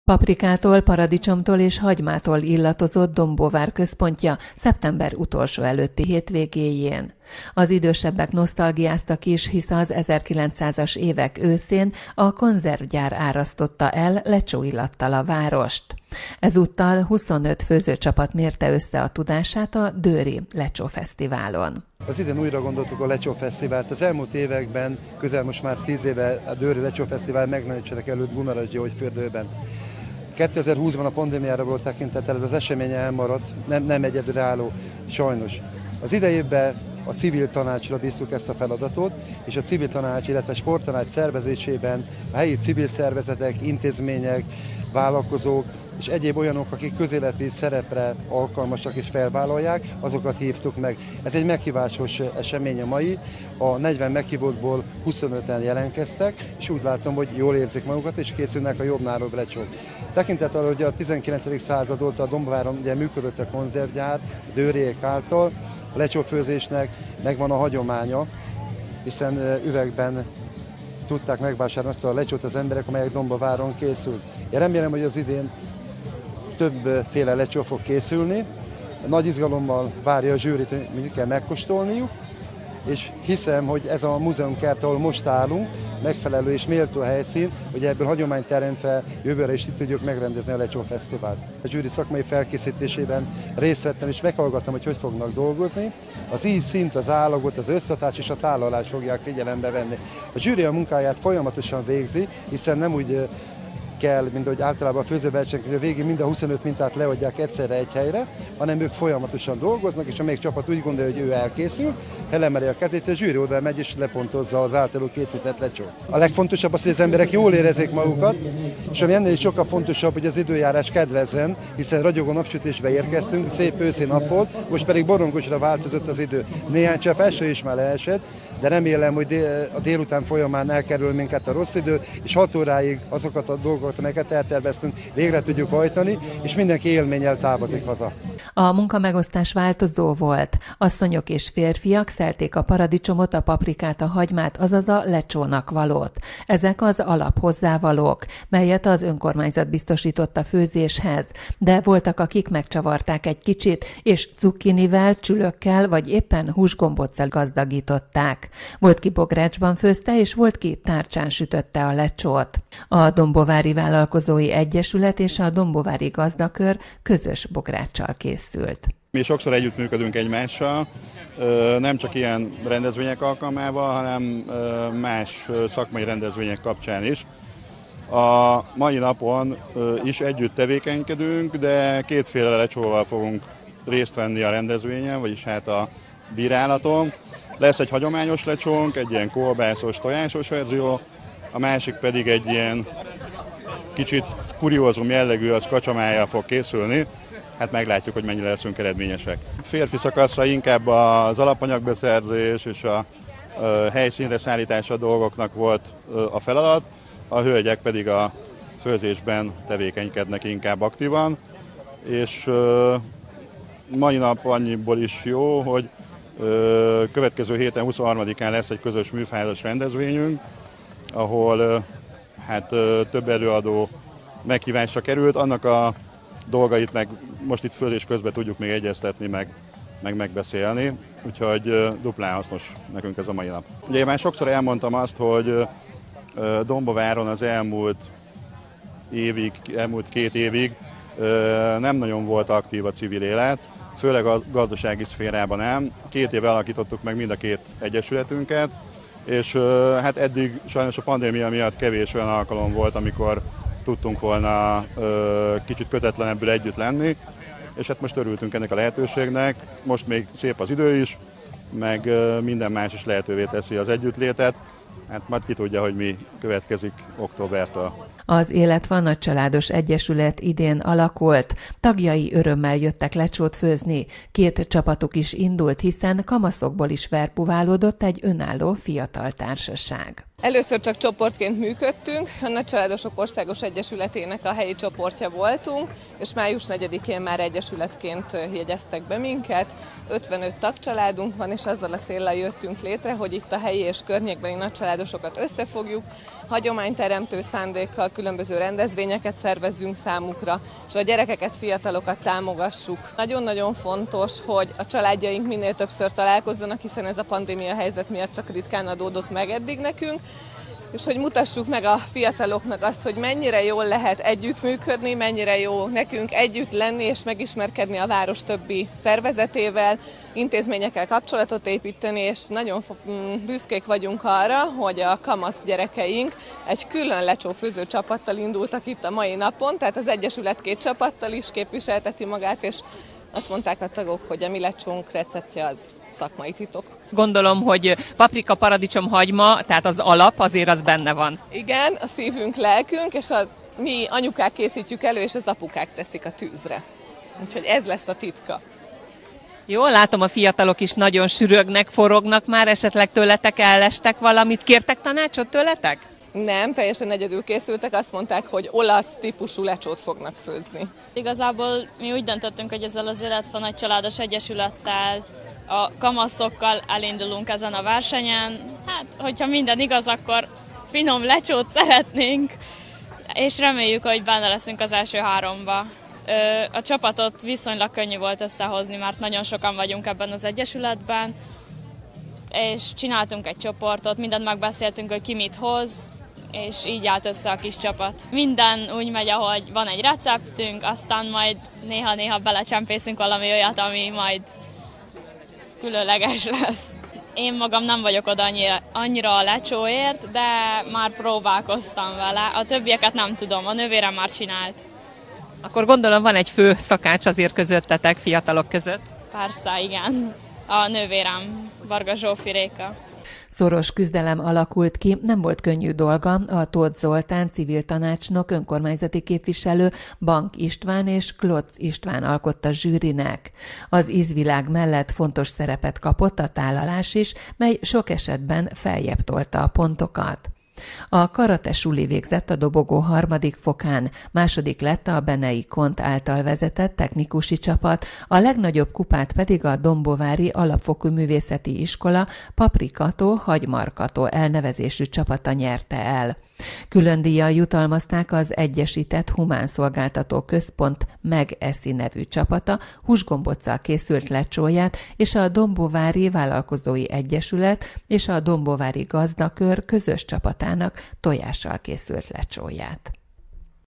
Dőry Lecsófesztivál - Dombóvár
A több évtízedes múlttal rendelkező Dőry Lecsófesztivált ezúttal nem Gunarason, hanem Dombóváron a Polgármesteri Hivatal mögötti Múzeumkertben tartották meg, meghívásos alapon. Ezúttal 25 csapat mérte össze főzőtudományát, mellyel az 1900-as években működött dombóvári Konzervgyár emléke előtt tisztelegnek.